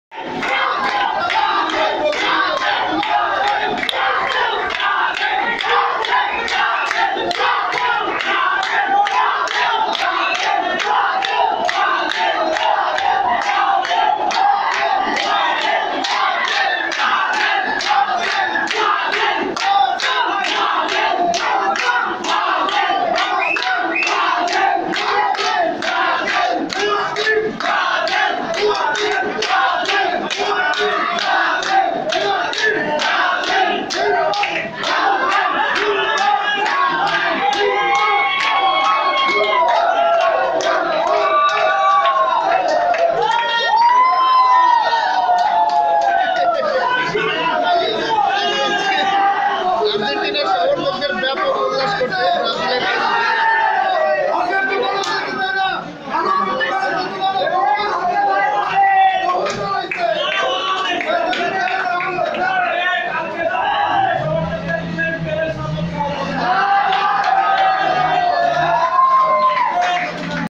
ব্রাজিল হারছে, বেলজিয়াম জিতছে, লাফাচ্ছে আর্জেন্টিনার সমর্থক | Brasil Lost, Argentina Fans Celebrate
ব্রাজিলের পরাজয়ে আর্জেন্টিনার সমর্থকদের উল্লাস